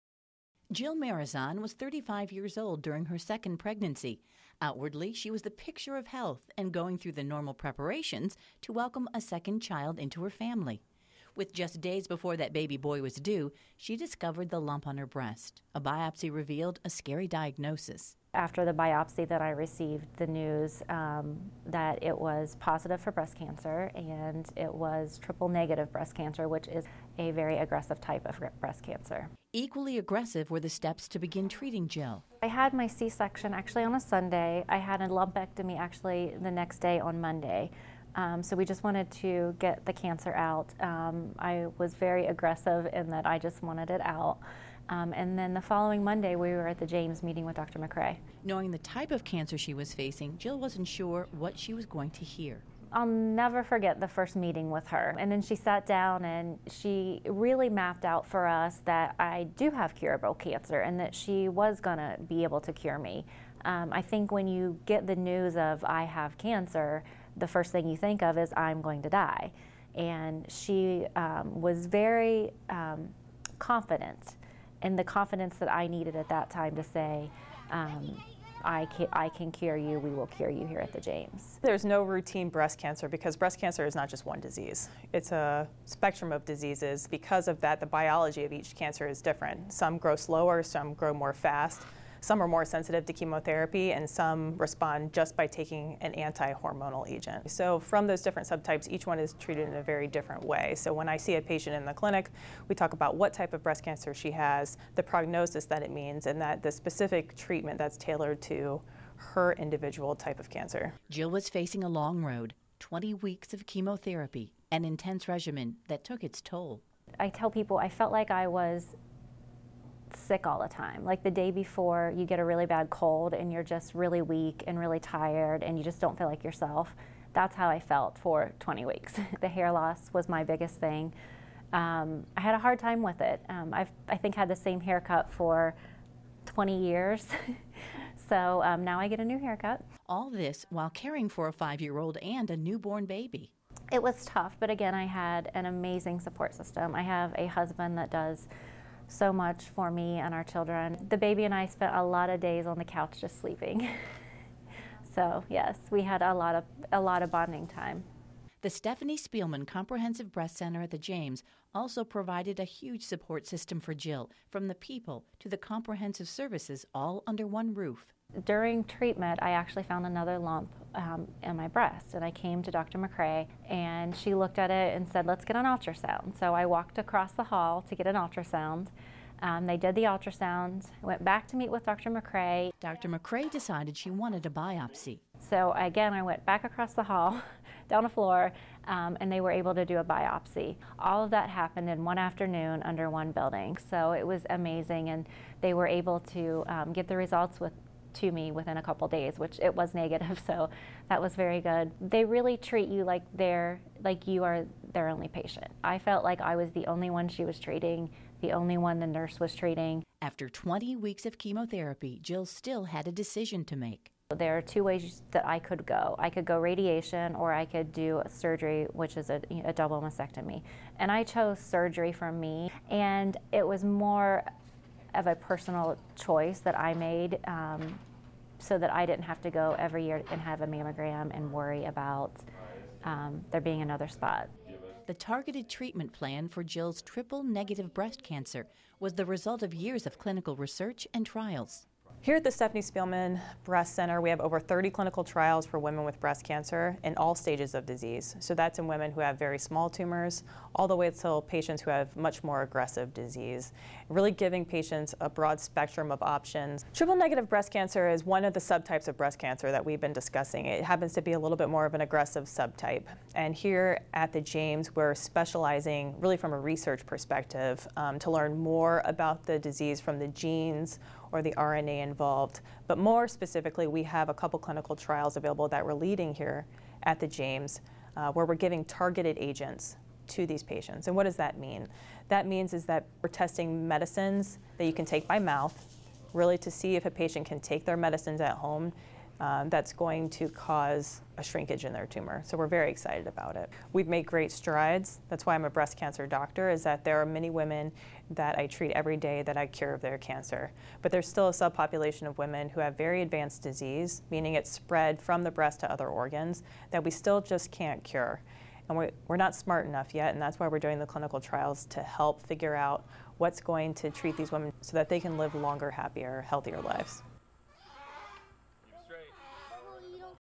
Me Time: The Importance of Women’s Health and Cancer
Breast Medical Oncologist Dr. Erin Macrae, explains the importance of preventive breast health and  what you can do to stay cancer free. Breast cancer patient Jyl Marazon shares her phenomenal story of surviving triumphantly in the face of tragedy.